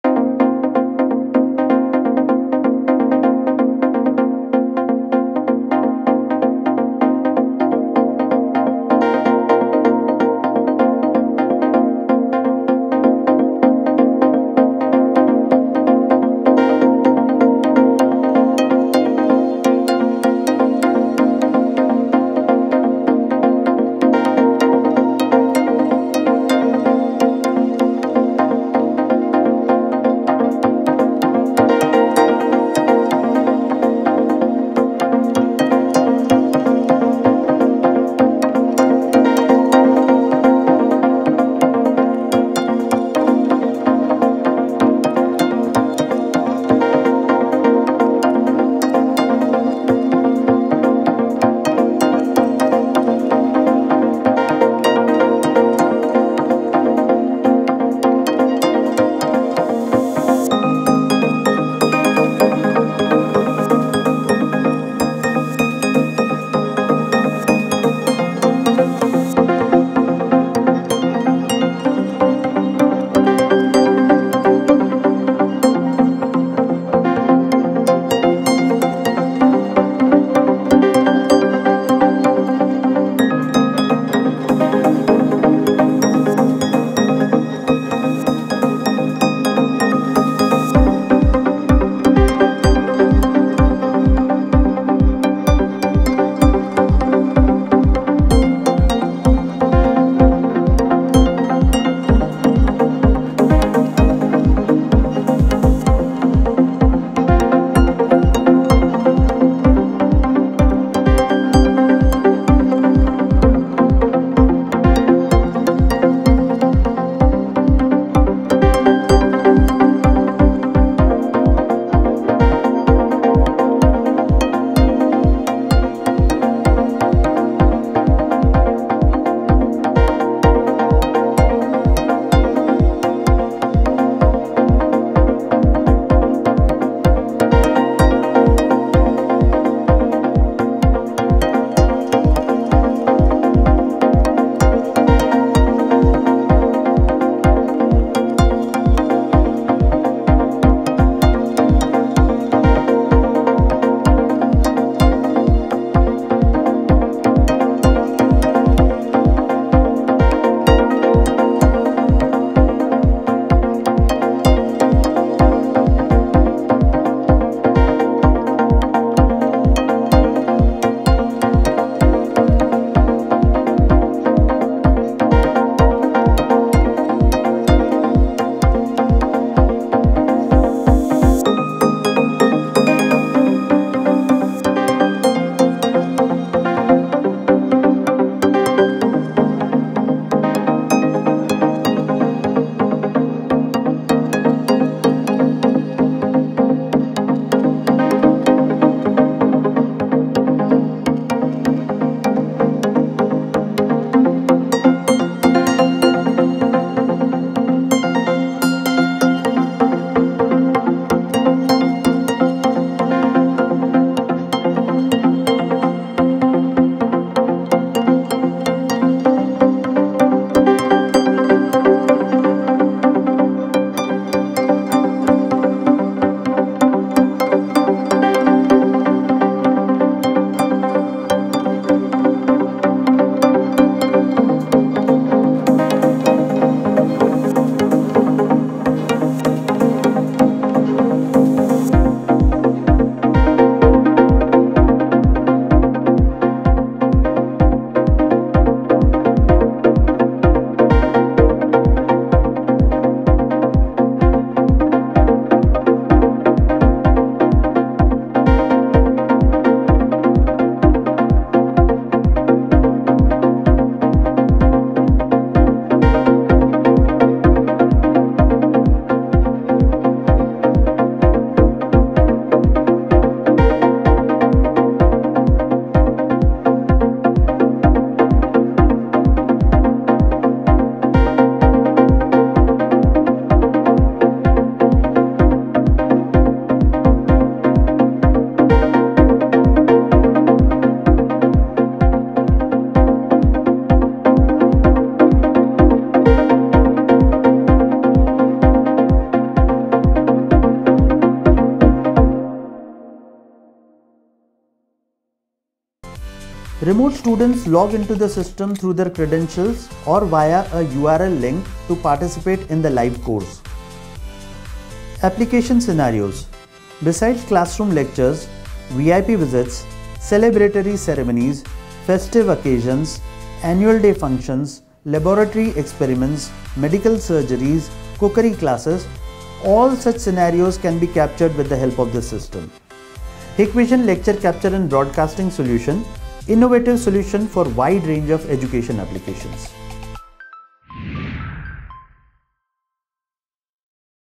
Hikvision Lecture Capture and Broadcasting Solution
Presenting Hikvision’s Lecture Capture and Broadcasting solution. This solution comprises a special Broadcasting DVR, a teacher tracking camera, a student capture camera, audio pickups and the classroom laptop. The DVR receives the inputs from all the devices, stitches the videos together, and provides a unique three screen output.

This solution aids online education by relaying lectures online in real time to students across the world. Besides broadcasting, the lectures are also saved centrally to create a knowledge hub within the institute.

This video clip also features our innovative Interactive Display Screen which is the future of writing boards.

Watch the amazing video for more details.